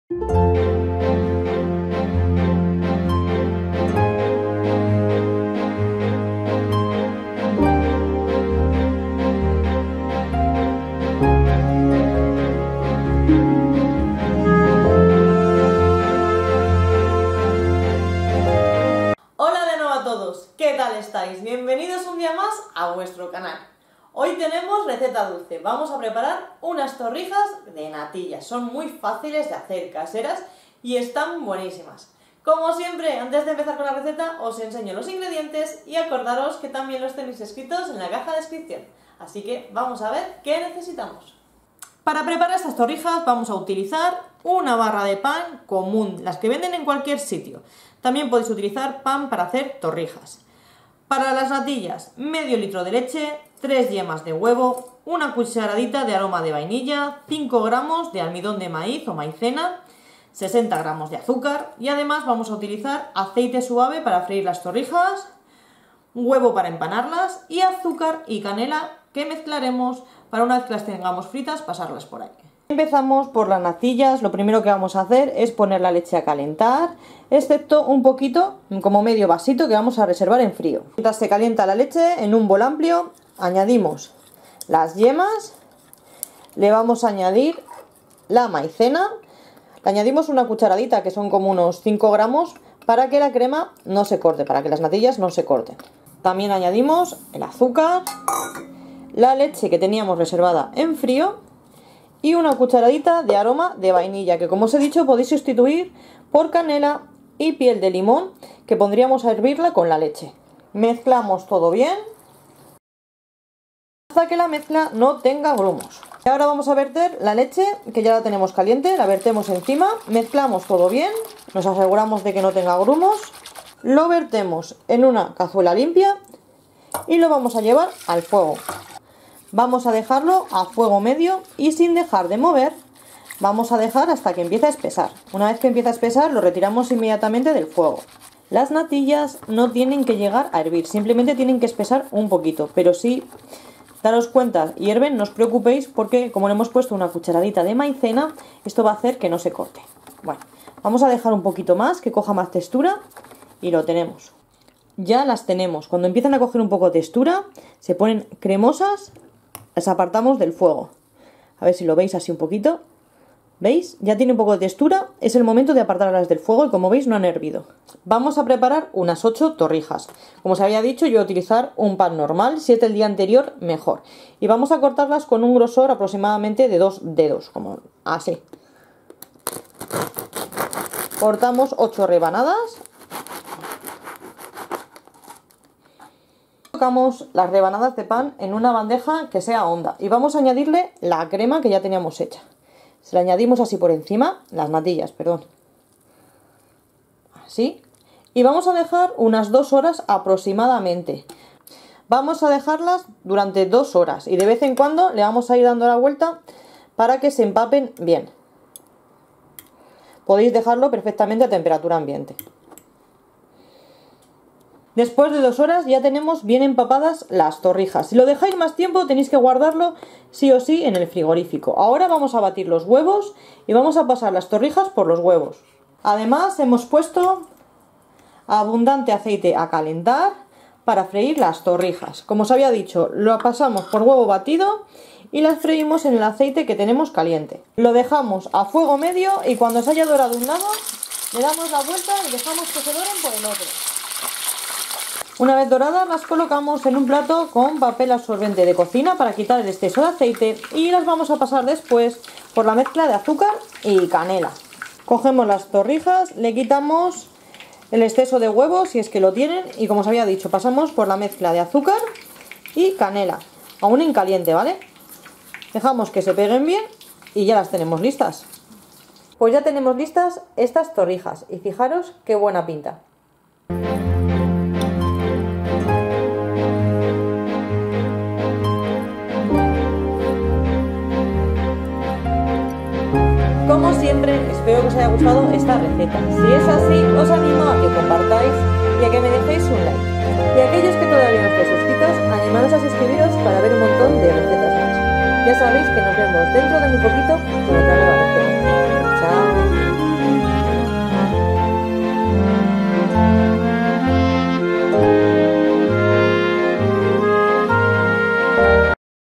💥TORRIJAS DE NATILLAS MUY FÁCILES Y BUENÍSIMAS
Las torrijas son un postre o un dulce muy típico de nuestro país y sobre todo en Semana Santa.

Hoy os dejo una nueva versión de este clásico, unas torrijas de natillas muy fáciles; están buenísimas, cremosas y además de sencillas son económicas y le gustan a todo el mundo.

Con esta receta te saldarán unas 8 unidades pero seguro que te saben a poco, te van a duran muy poco porque están deliciosas.

TIENDA ON LINE encontrarás los productos que uso en este vídeo

INGREDIENTES PARA UNAS 8 UNIDADES:

1 barra de pan común o pan para hacer torrijas
500 ml de leche entera
3 yemas de huevo
60 gr de azúcar común
5 gr de maizena o harina fina de maíz
1 cucharadita de aroma de vainilla
Huevos batidos
Aceite de freír
Azúcar y canela en polvo

Si preparas alguna de mis recetas comparte en Instagram y no olvides etiquetarme @cocineraymadre